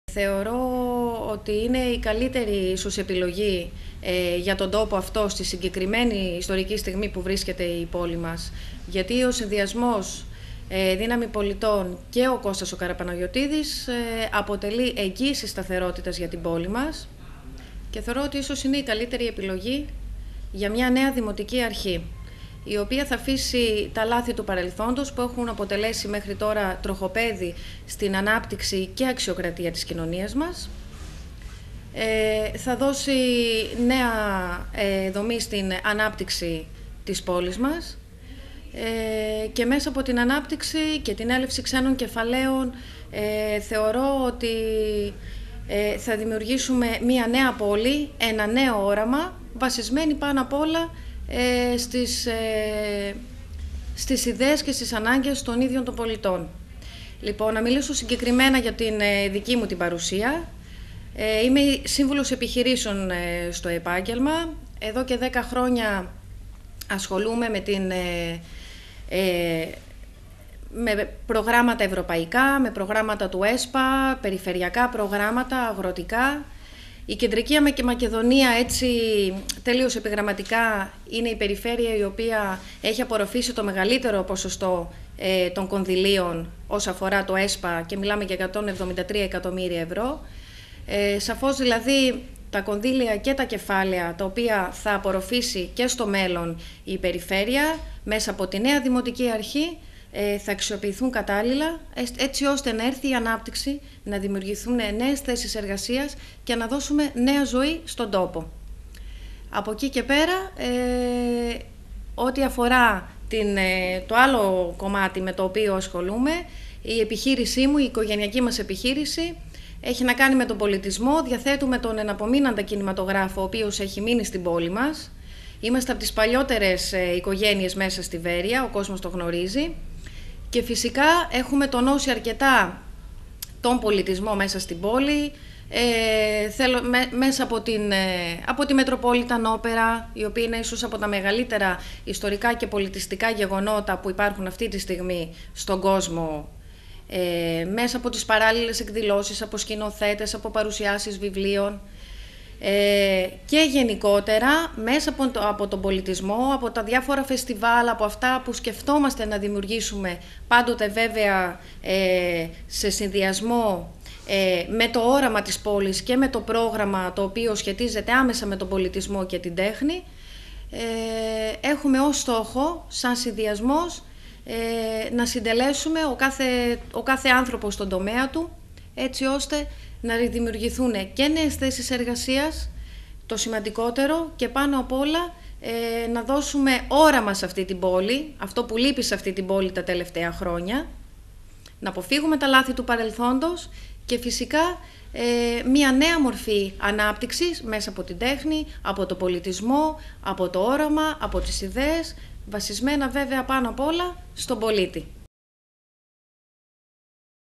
ΙΦΙΓΕΝΕΙΑ ΒΛΑΧΟΓΙΑΝΝΗ
Την ανάγκη μιας νέας πόλης, ενός νέου οράματος βασισμένο στις ανάγκες και τις επιθυμίες των πολιτών επισημαίνει η υποψήφια δημοτική σύμβουλος Βέροιας  με τον συνδυασμό του Κώστα Καραπαναγιωτίδη «Δύναμη Πολιτών» Ιφιγένεια Βλαχογιάννη. Παράλληλα, η κα. Βλαχογιάννη χαρακτήρισε ως εγγύηση σταθερότητας την επιλογή του συνδυασμού του κ. Καραπαναγιωτίδη για την πόλη της Βέροιας.